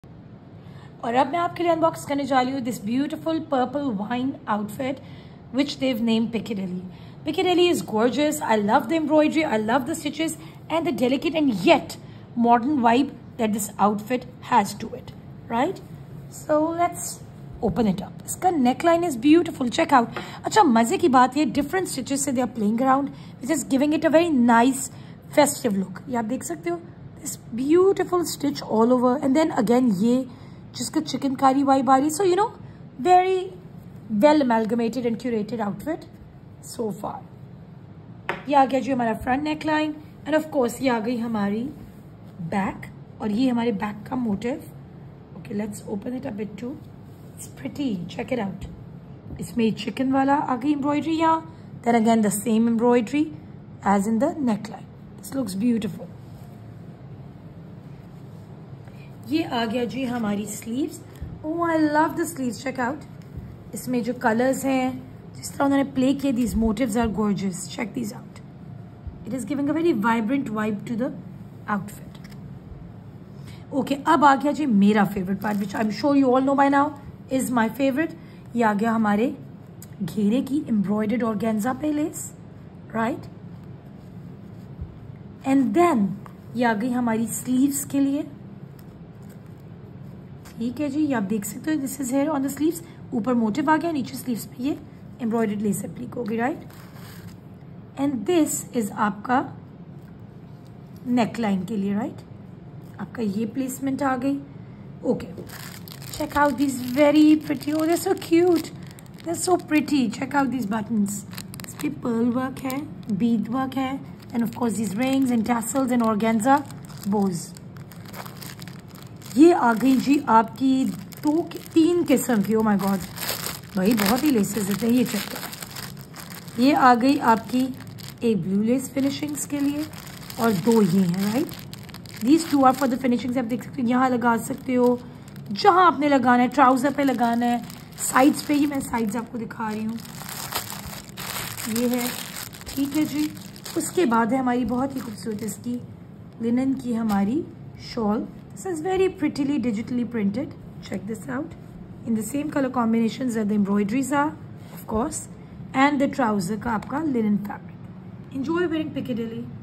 Broadway Showtime | Unstitched Winter Edit ’23 | PRE-BOOK NOW
Piccadilly | Rs. 12,950

In-Stores Pre-Booking Starts on 16th October

Elevate your style with our "Piccadilly Charm" ensemble, a perfect blend of sophistication and playful allure. The embroidered linen shirt, adorned with intricate detailing, exudes timeless elegance. Paired with a printed linen shawl featuring delicate crochet lace finishing’s and a bow dori tassel, this ensemble is a celebration of chic style and artistic finesse.